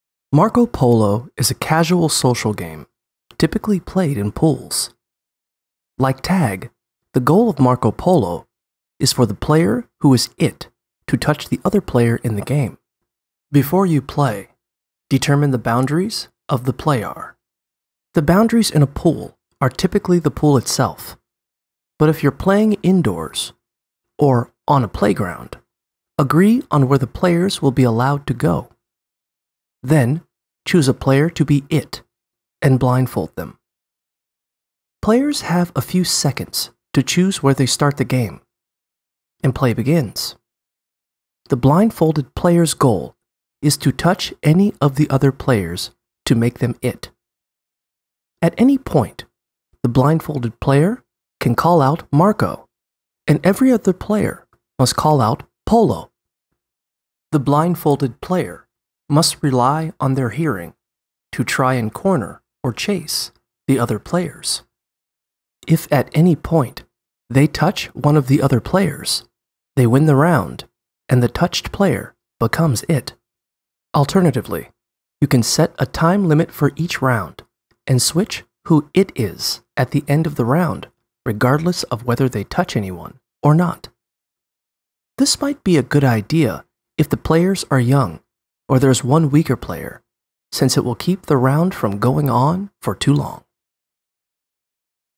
How to Play Marco Polo
Learn how to play marco polo with this guide from wikiHow